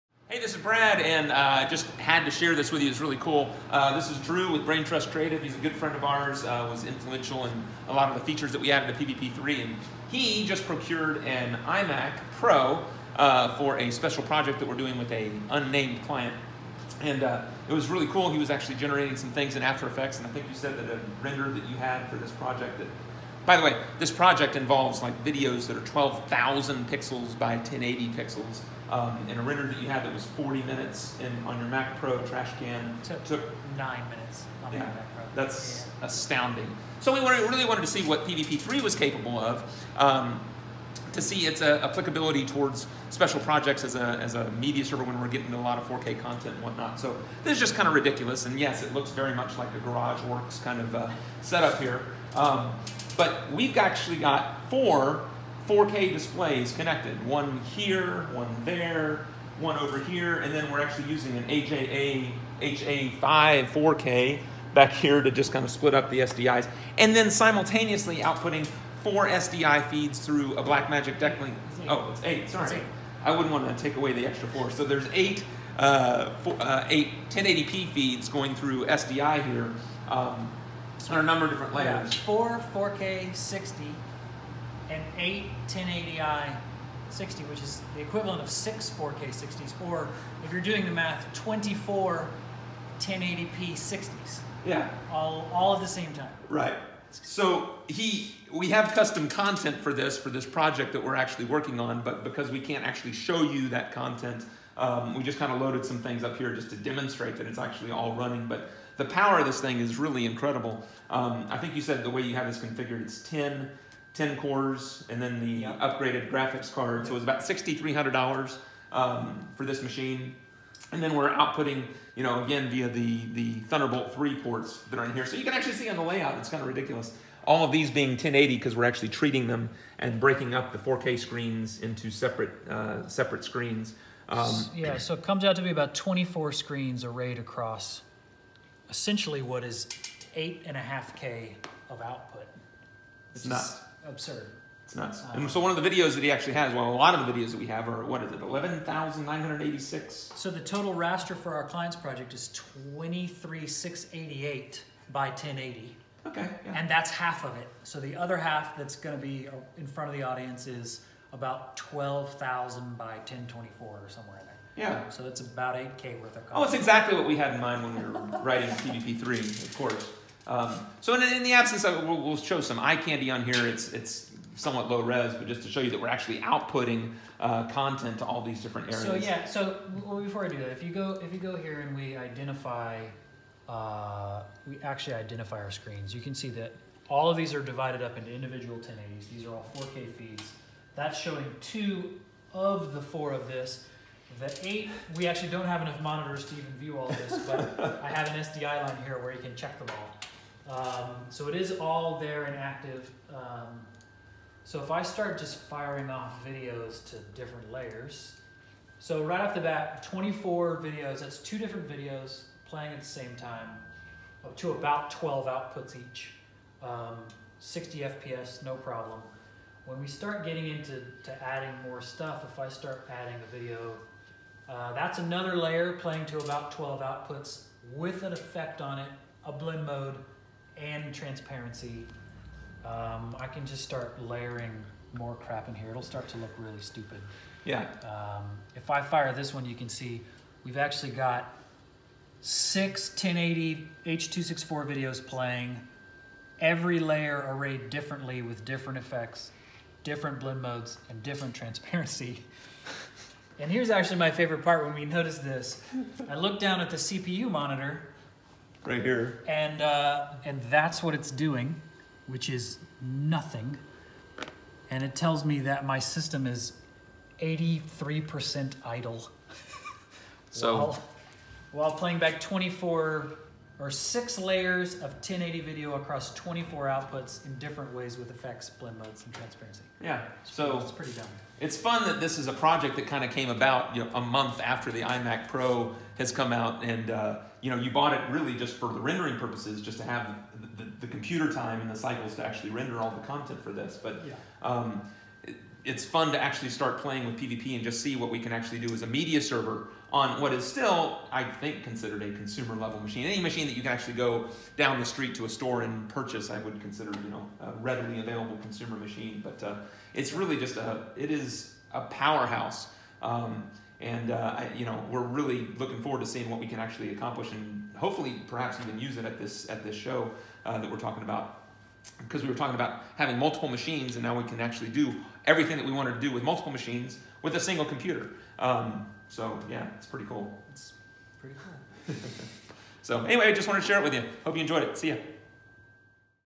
PVP3 and iMac Pro
A quick demonstration of just how powerful PVP3 is on the new iMac Pro.